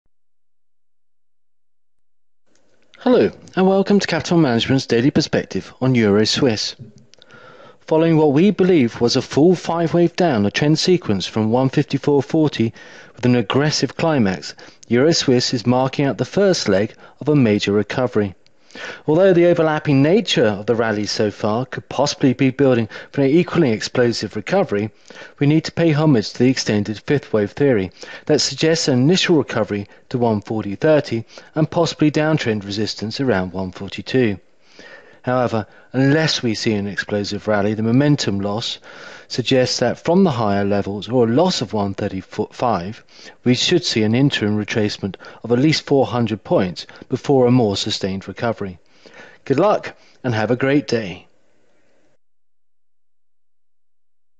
Forex Trading Strategy on Euro Swiss - Classic Elliott Wave Reversal!
Euro Swiss is marking out the first leg of a major recovery along the lines of classic extended fifth wave theory.